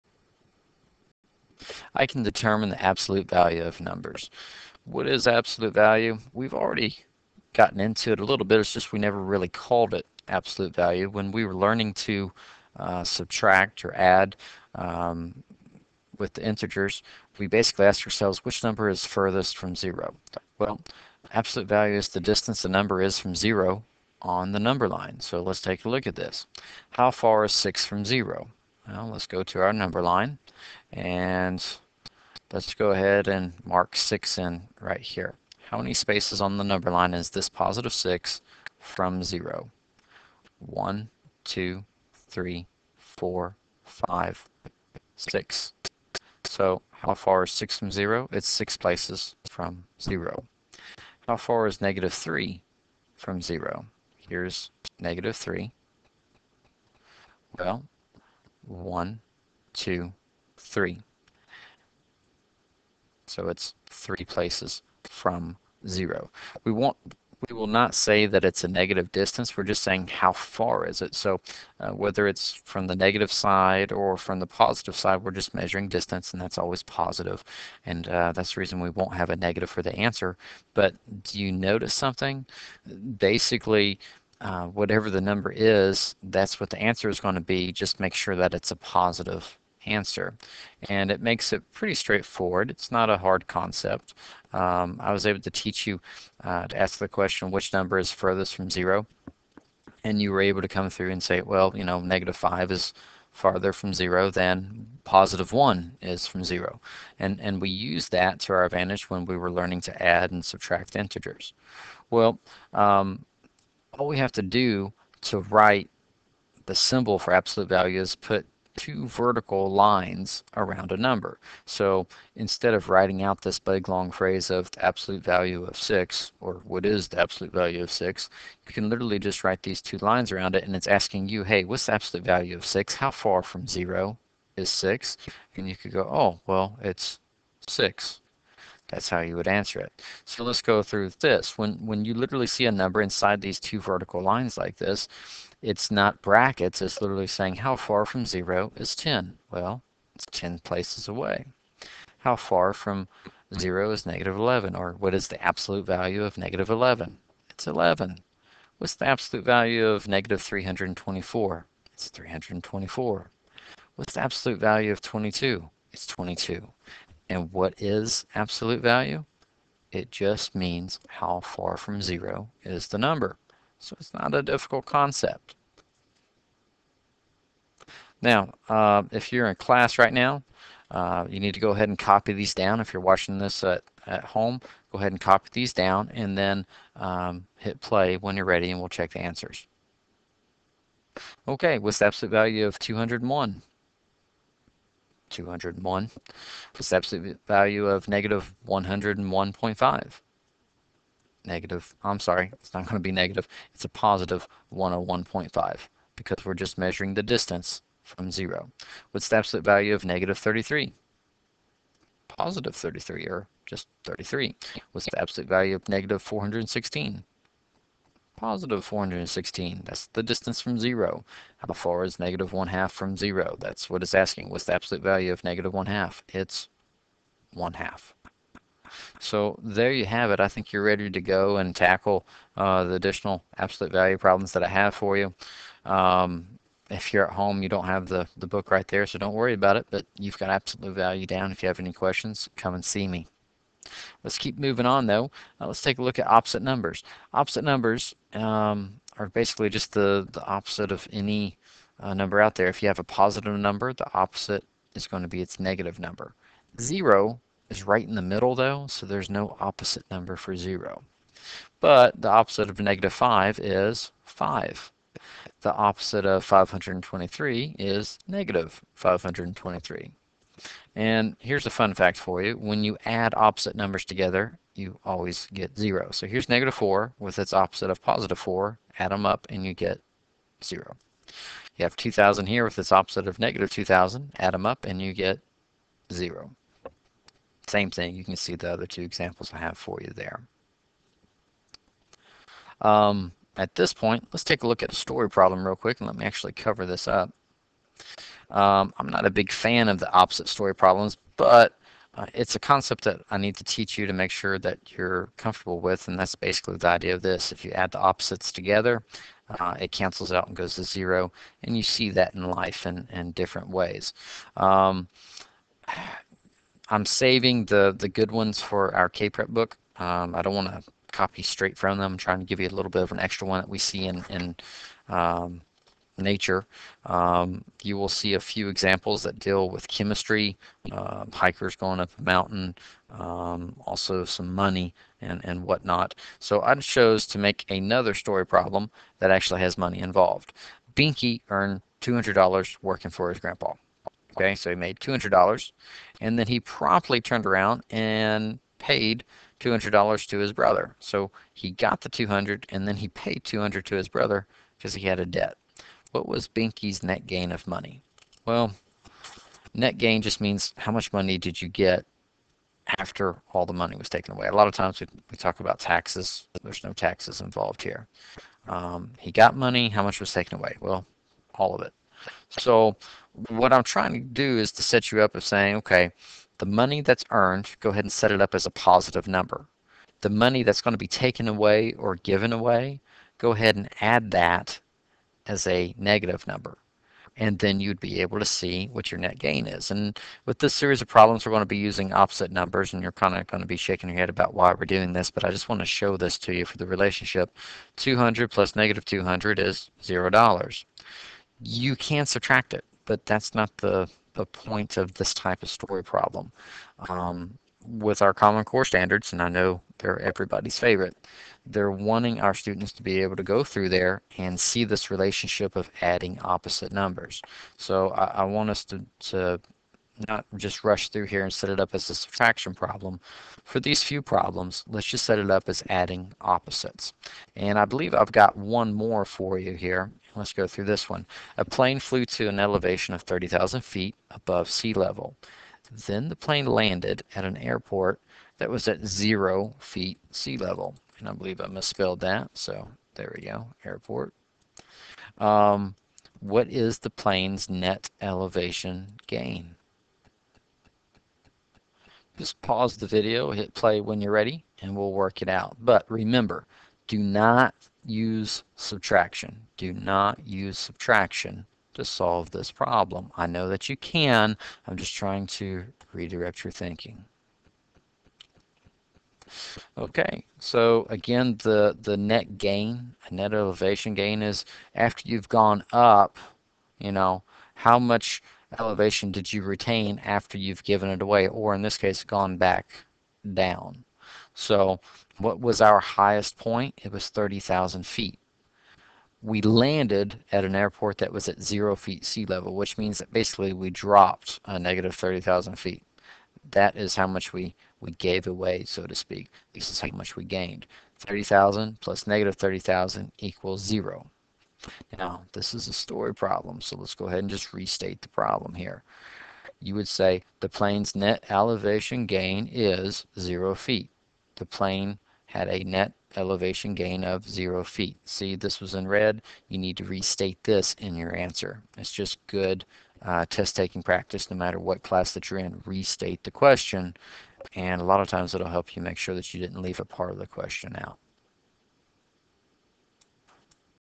4 - absolute value and opposite numbers
Basic overview of absolute value.  Then we start treading into opposite numbers.  Hit upon the idea that the sum of opposite numbers is always zero.